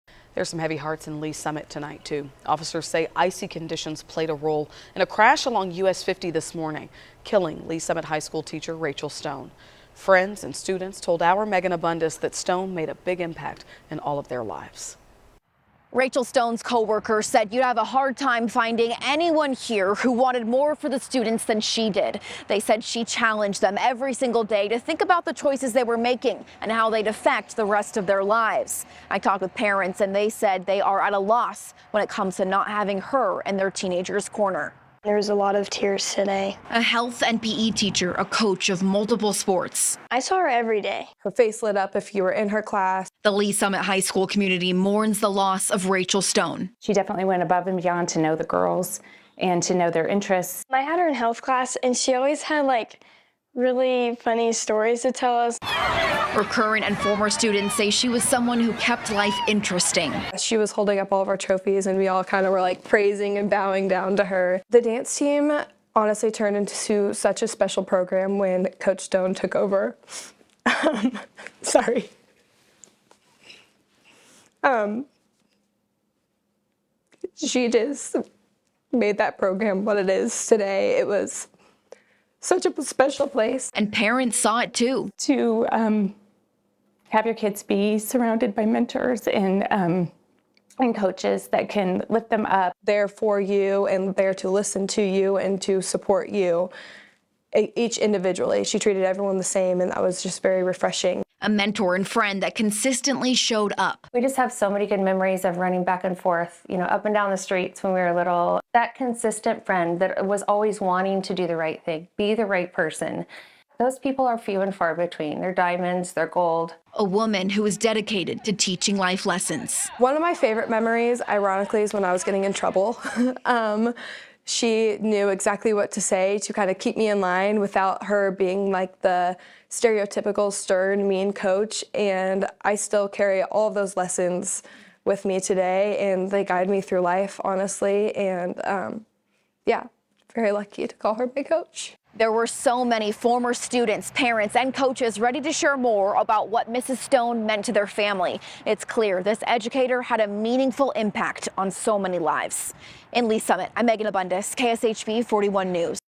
Students remember teacher, coach killed Thursday in wreck on icy road
Rachel Stone’s coworkers said it’s hard to find anyone who wanted more for their students than Stone did.